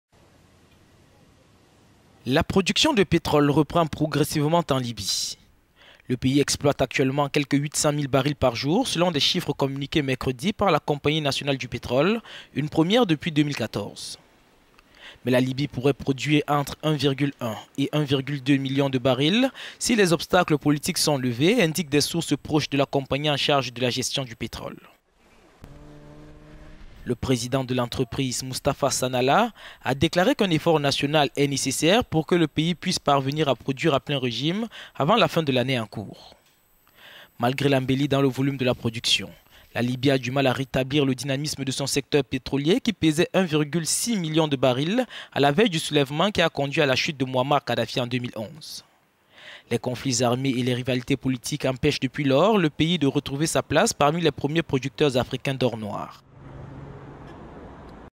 Libye : la production atteint 800 000 barils par jour
La production de pétrole reprend progressivement en Libye. Le pays exploite actuellement quelque 800 000 barils par jour, selon des chiffres communiqués mercredi par la Compagnie nationale du pétrole. Une première depuis 2014.

Mais la Libye pourrait produire entre 1,1 et 1,2 million de barils si les obstacles politiques sont levés, indiquent des sources proches de la compagnie en charge de la gestion du pétrole.

Le président de l'entreprise Mustafa Sanalla a déclaré qu'un effort national est …